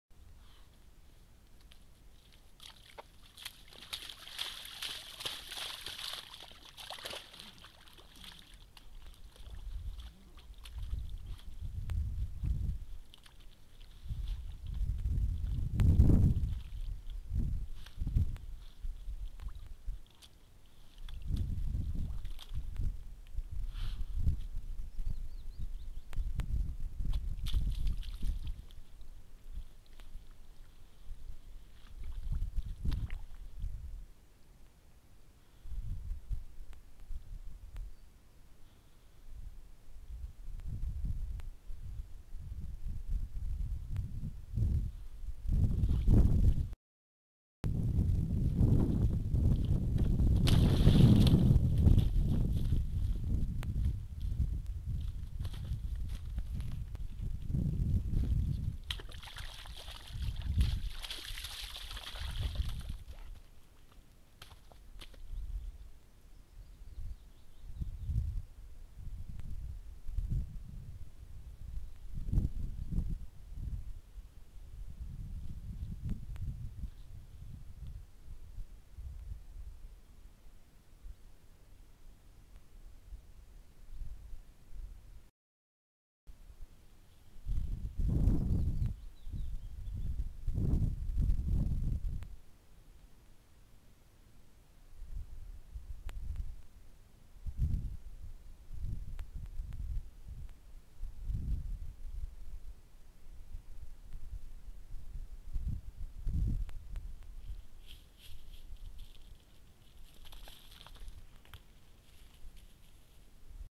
Hjortekolla i badegrop / red deer in a bathing pit 08.07.16
Satte opp viltkamera ved ei badegrop, litt dårlig vinkel. Satt sammen noen opptak av ei spenstig ungkolle som ser ut til å kose seg :)